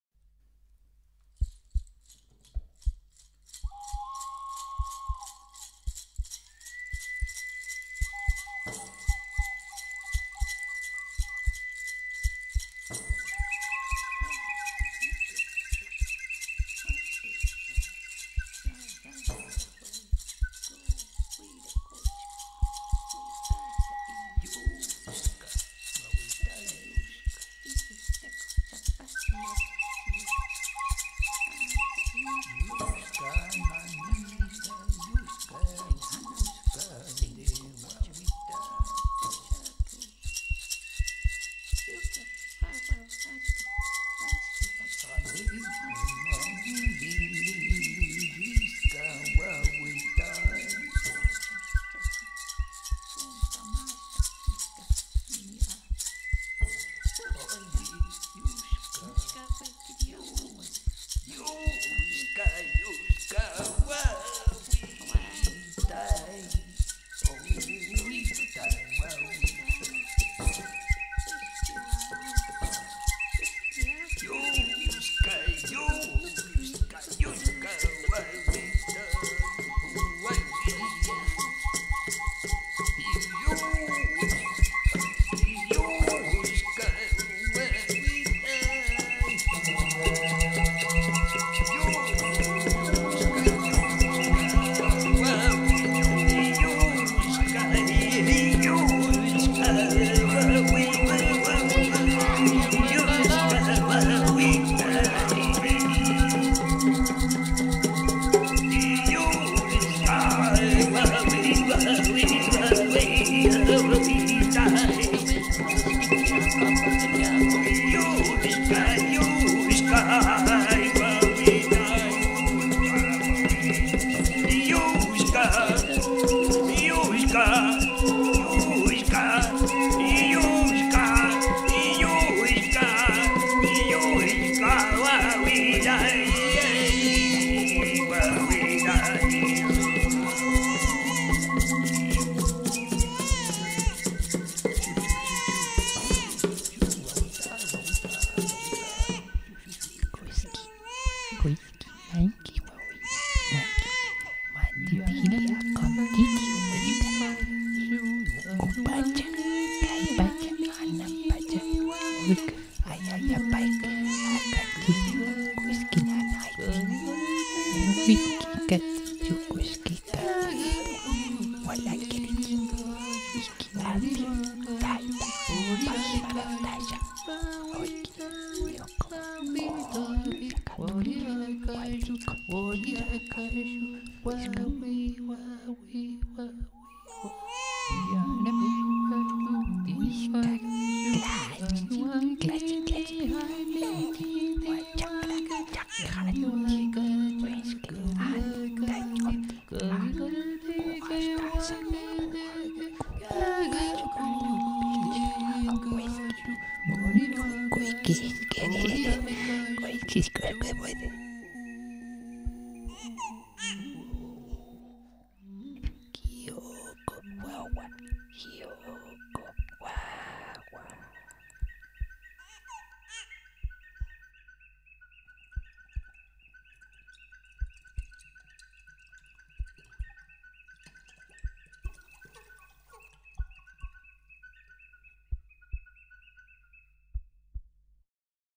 Paisaje Sonoro - Parto Moche
En este paisaje sonoro intento recrear lo que habría sido el parto de un bebé de la alta sociedad de la cultura Moche del Área intermedia andina (200-800 d.C)
Los personajes sería un curandero, una matrona, el padre, varios músicos y el bebé (guagüitay). La madre que está pariendo serías tú en primera persona.

Los sonidos son de corazón y diversos instrumentos: Tambor de cerámica, trompetas moche, vasijas silbadoras, flauta poliglobular, sonajas, canto.

Esto nace de un imaginario, un ejercicio, en ningún caso asegura que así hayan sido estas experiencias.

RECOMIENDO AUDÍFONOS!